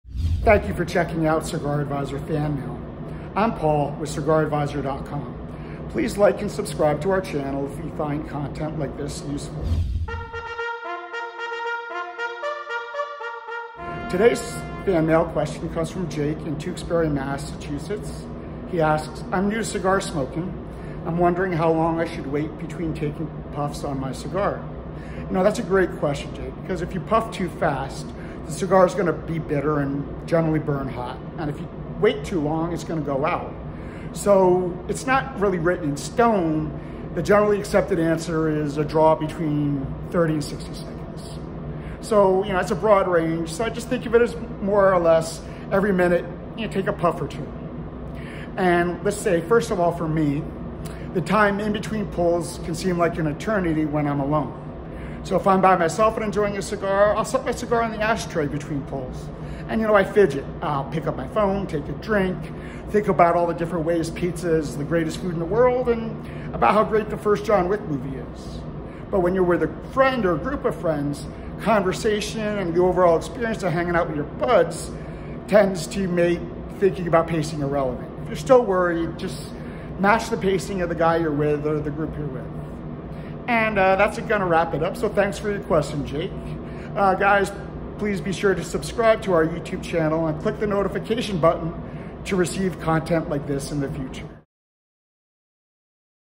How Long Between Draws On Your Cigar? - Cigar 101
Ever wonder if there’s right pace for enjoying a cigar? Smoke too fast and it burns hot, and the flavors get bitter. Too slow and it’ll go out and you must relight. Check out this quick explanation by Cigar Advisor Paul and discover how to enjoy a cigar like a pro.

Follow the Cigar Advisors to stay up to date on everything cigars.